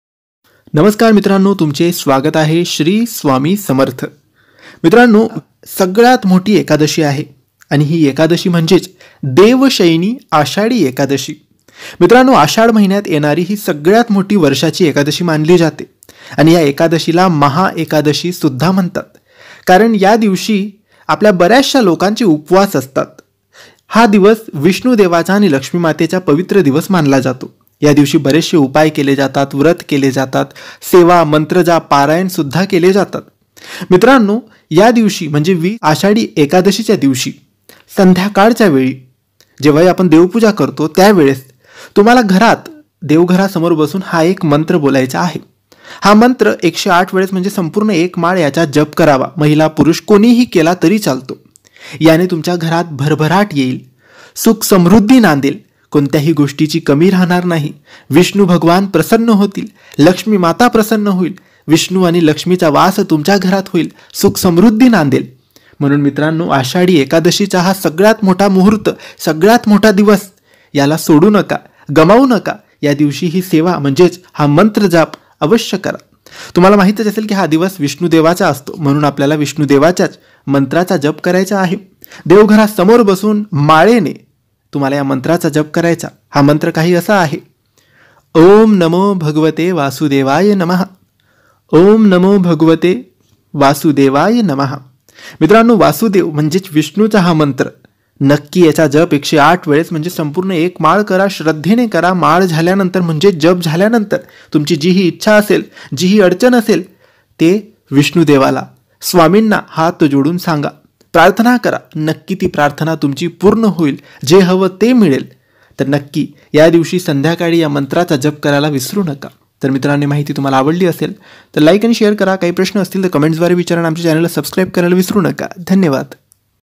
29 जुन मोठी आषाढी एकादशी संध्याकाळी बोला हा चमत्कारि मंत्र भरभराट येईल shree swami samarth
29 जुन मोठी आषाढी एकादशी संध्याकाळी बोला हा चमत्कारि मंत्र भरभराट येईल shree swami samarth